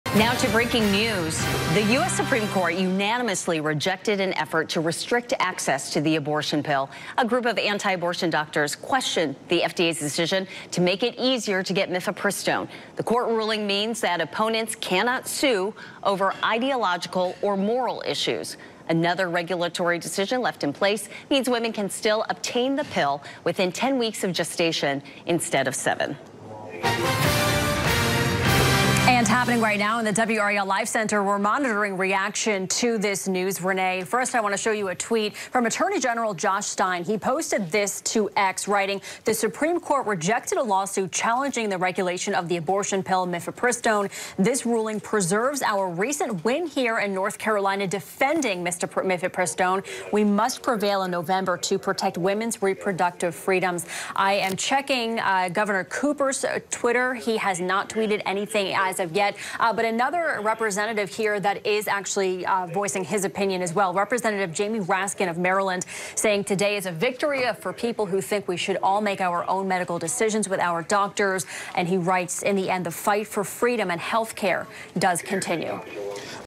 Unanimous Supreme Court keeps Mifeprisone on the market; Lawmakers react
The Supreme Court on Thursday unanimously preserved access to a medication that was used in nearly two-thirds of all abortions in the U.S. last year, in the court’s first abortion decision since conservative justices overturned Roe v. Wade two years ago.

The nine justices ruled that abortion opponents lacked the legal right to sue over the federal Food and Drug Administration’s approval of the medication, mifepristone, and the FDA's subsequent actions to ease access to it. The case had threatened to restrict access to mifepristone across the country, including in states where abortion remains legal.